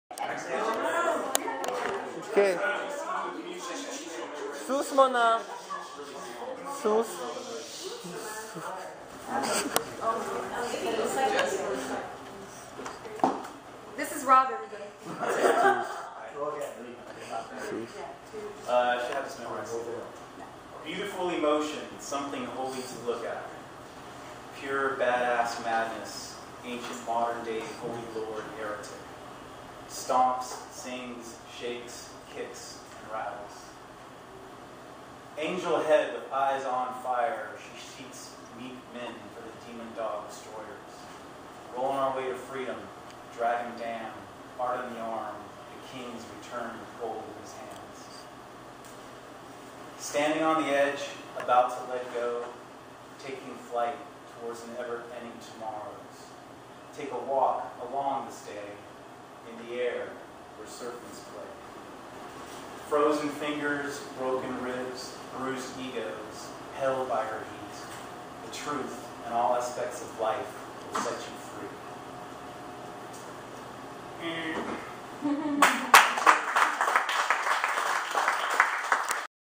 Rob Chevelle from Lit Soul
Live reading from Rob Chevelle of Lit Soul at the SoapBox Poets Open Mic at the Green Carrot Coffee House.